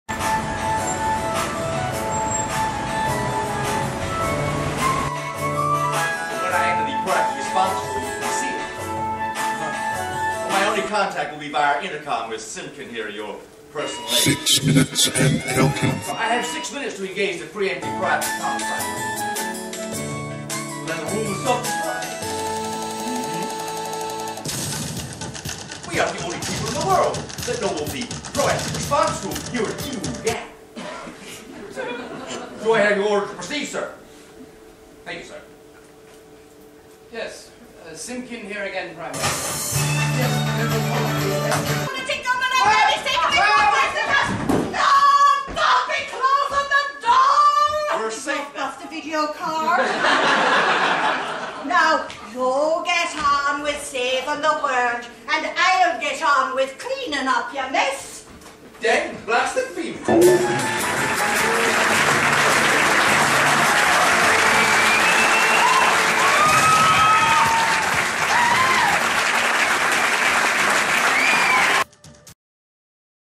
Six Minutes and Counting
Opening Night of "Six Minutes and Counting" at the Seymour Centre for Short and Sweet 10 Minute Play Festival. A peek behind the scenes and a glimpse of the first hilarious performance on stage. Stars: Christine Greenough, Leofric Kingsford-Smith and Nick Mackenzie.